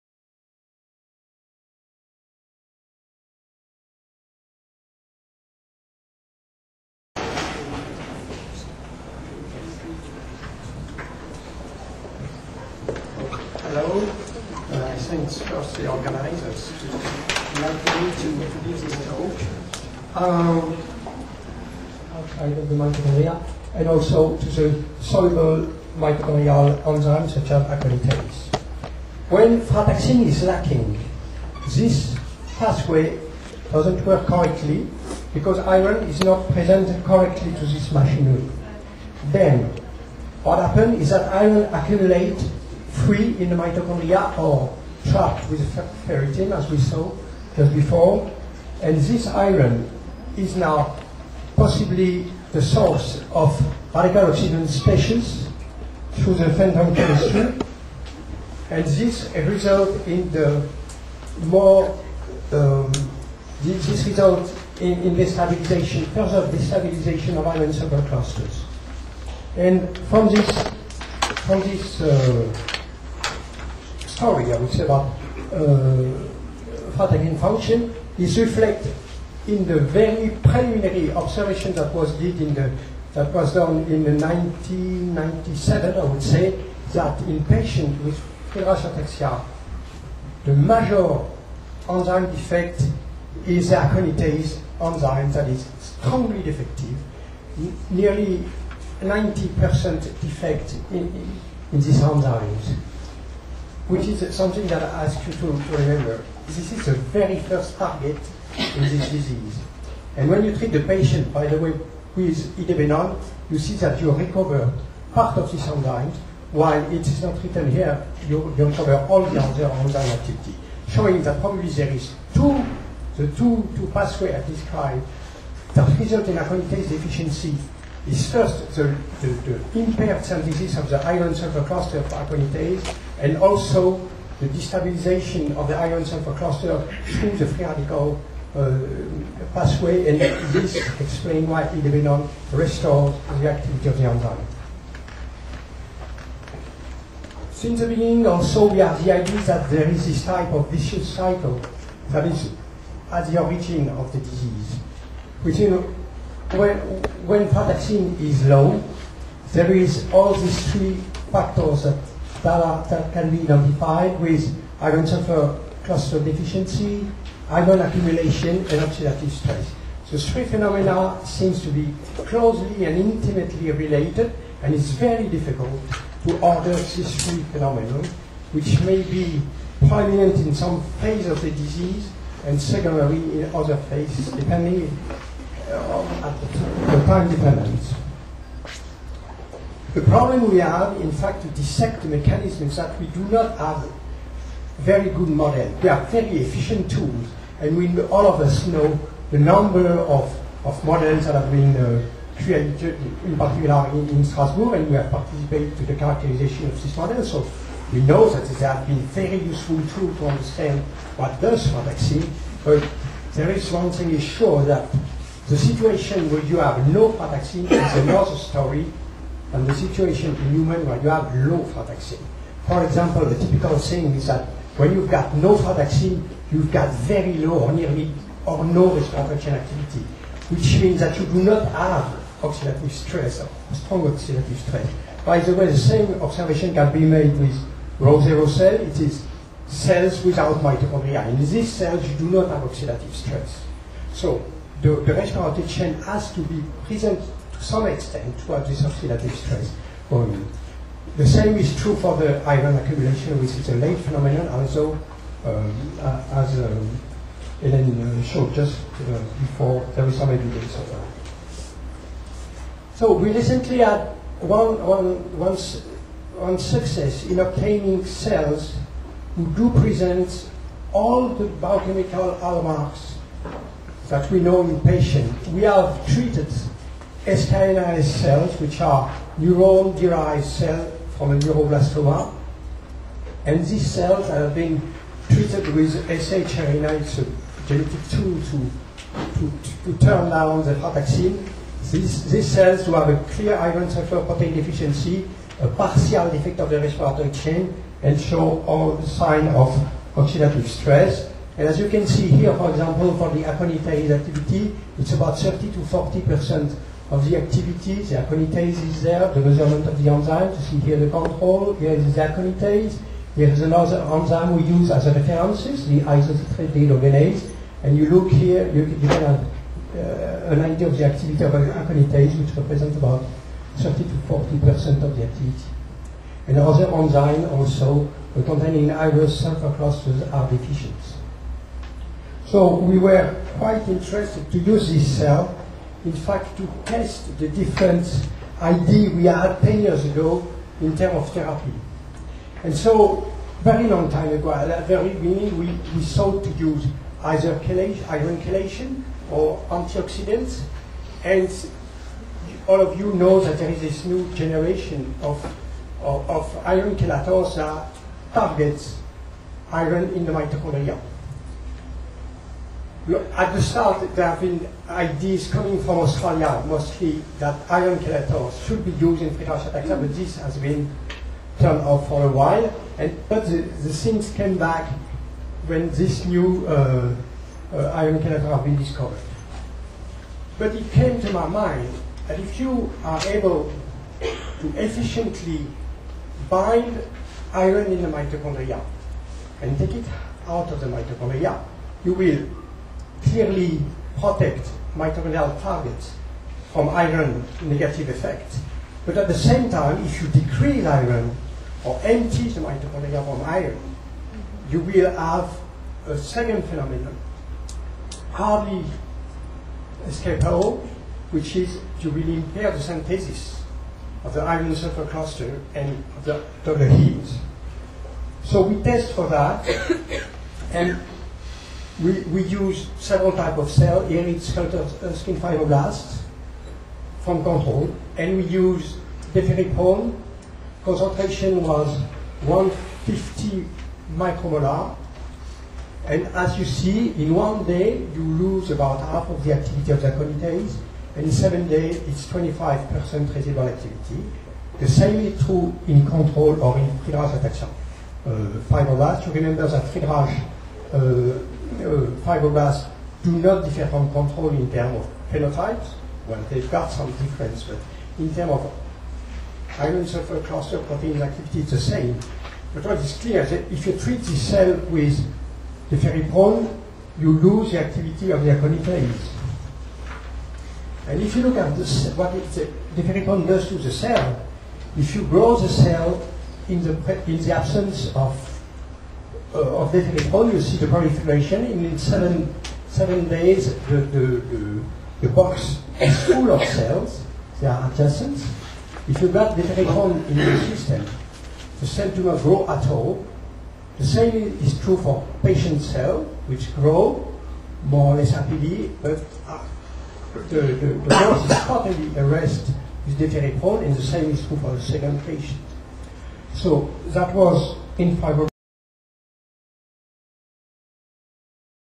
Dr. Pierre Rustin - part 1/2
Defining sensitive pathways and at risk targets in Friedreich Ataxia -
Dr. Pierre Rustin, Euro-ATAXIA Meeting -- Paris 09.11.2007
PART ONE